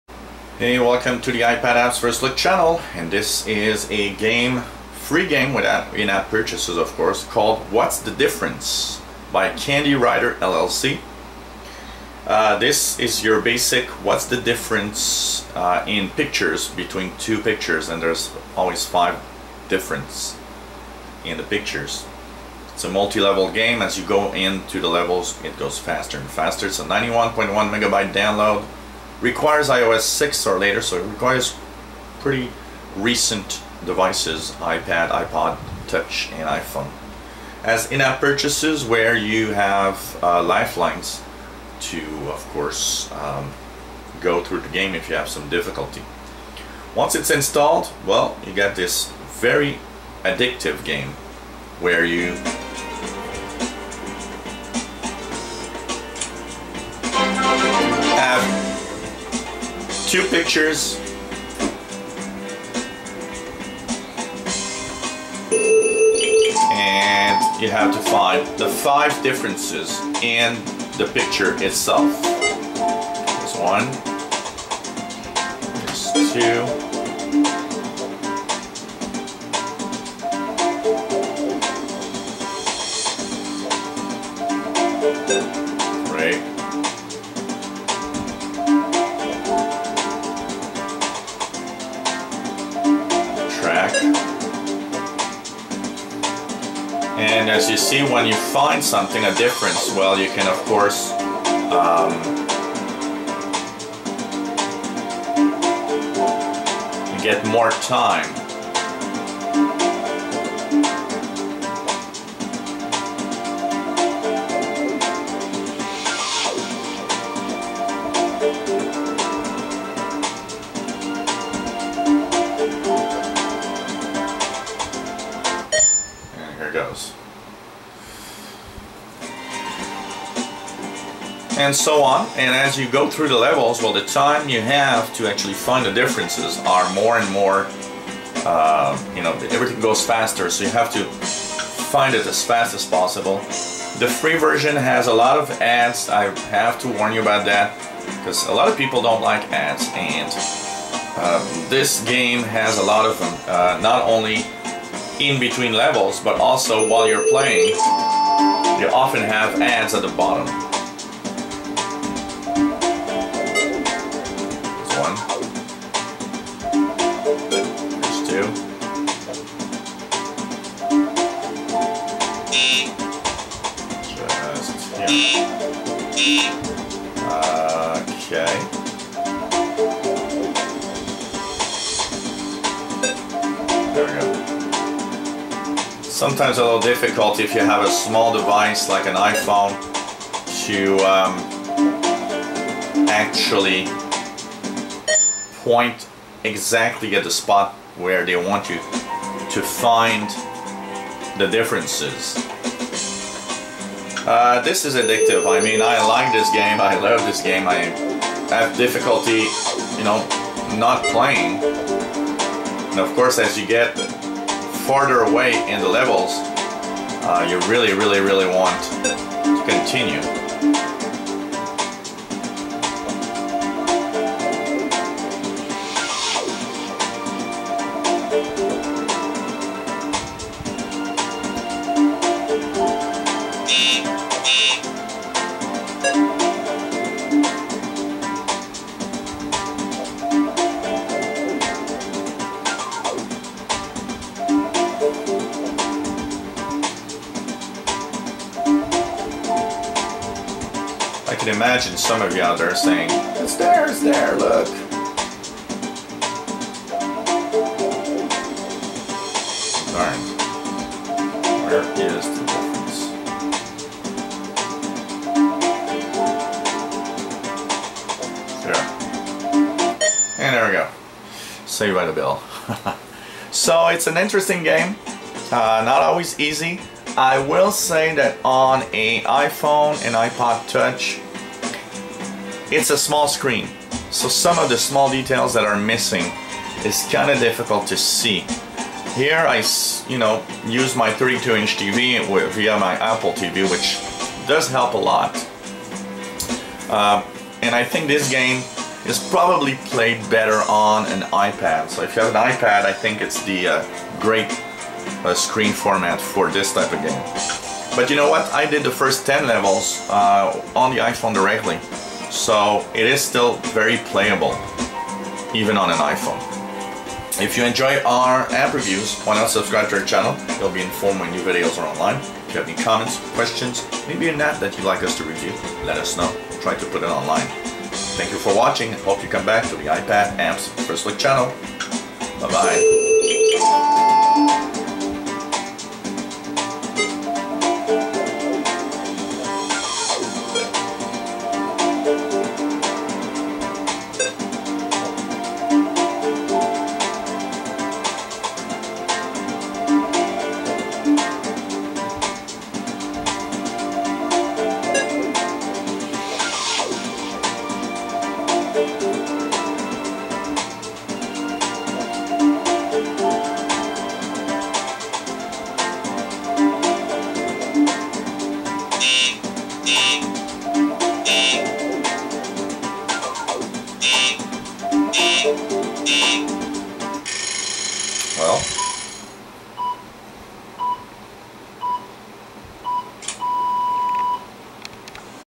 Ipad Iphone What's the difference game review
Nice addictive game where you have to find the 5 differences between 2 pictures